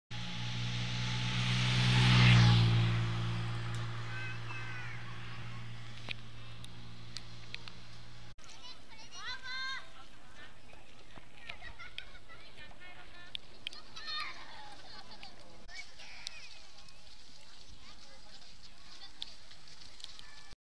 屈斜路湖 湖畔 砂湯
屈斜路湖湖畔の砂湯温泉　正面は屈斜路湖の中島